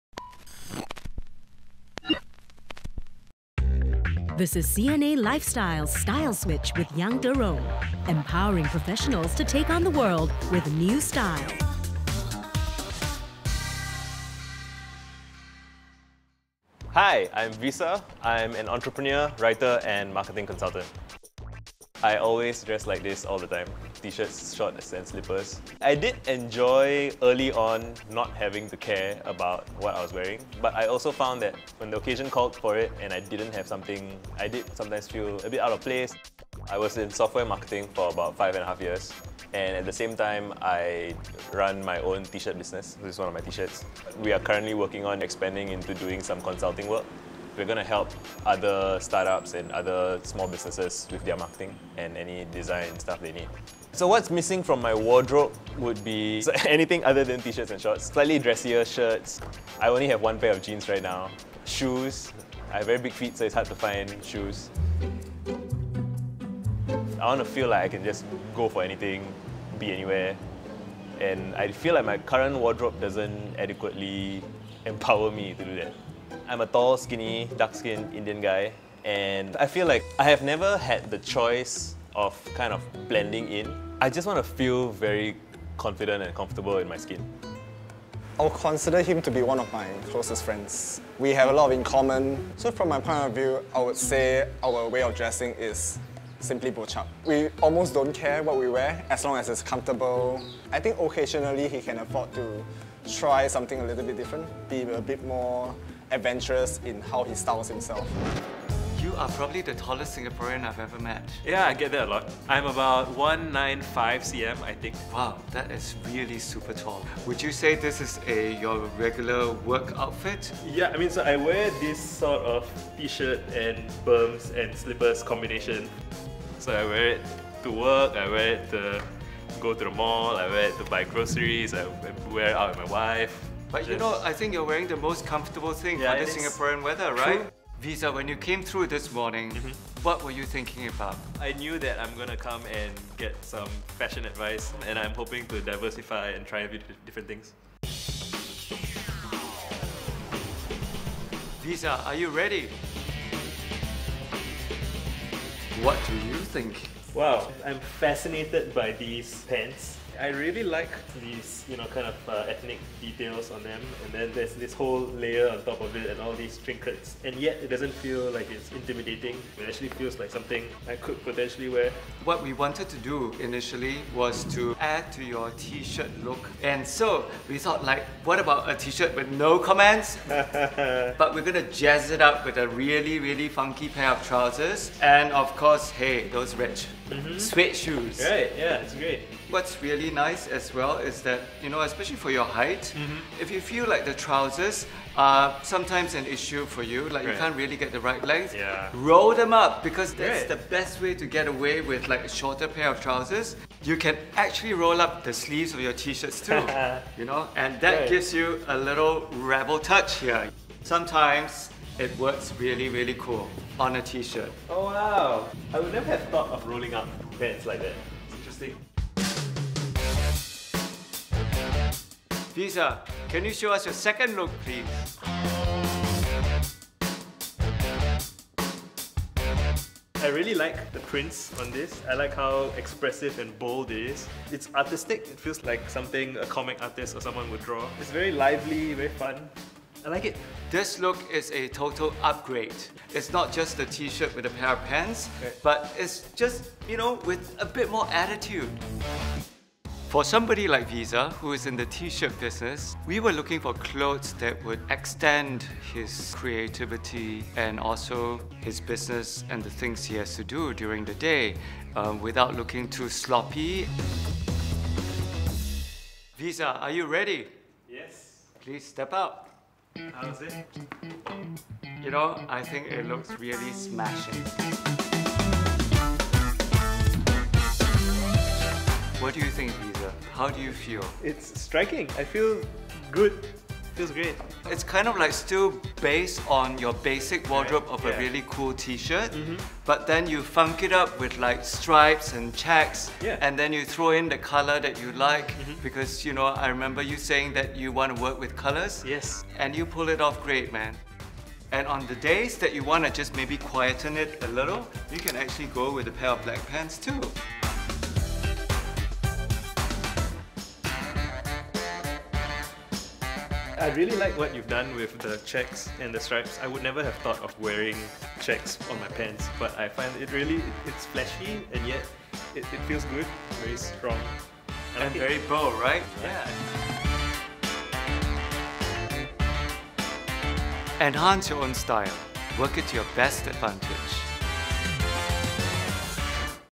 Style Switch: Making off-the-rack fashion work for this lanky, casual dude | CNA Lifestyle
Entrepreneur Visakan Veerasamy is 1.92m tall and has a habit of wearing berms and tees. CNA Lifestyle got him to trade these for wardrobe that’s sure to empower his creative pursuits.